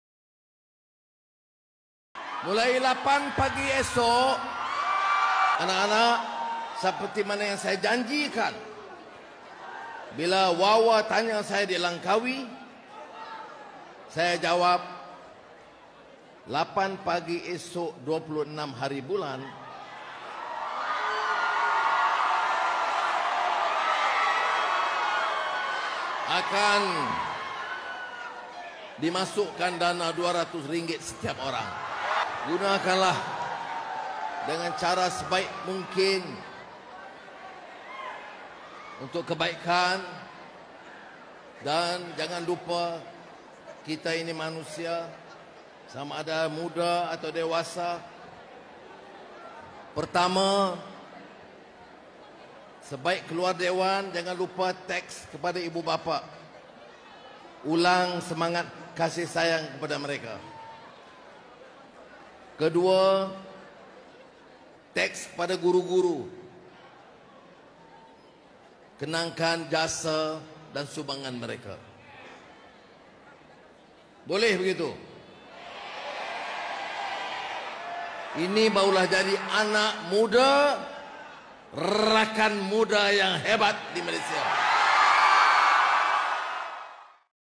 Program Ebelia Rahmah Malaysia Madani
Belia IPT 18-20 akan dikreditkan RM200 bagi program eBelia Rahmah Malaysia Madani